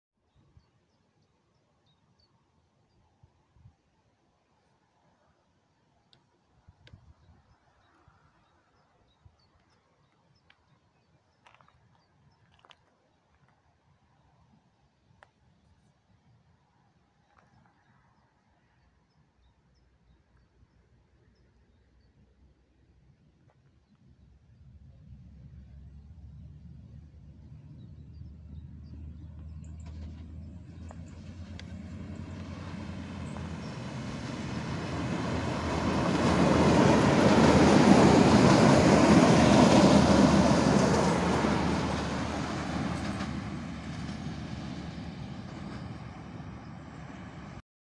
Pociąg Nr 77237 SA139-032 do Guben | Krosno Odrzańskie
O POCIĄGU:
Nazwa: SA139
Trasa:  Zielona Góra Główna → Guben
Wagony:brak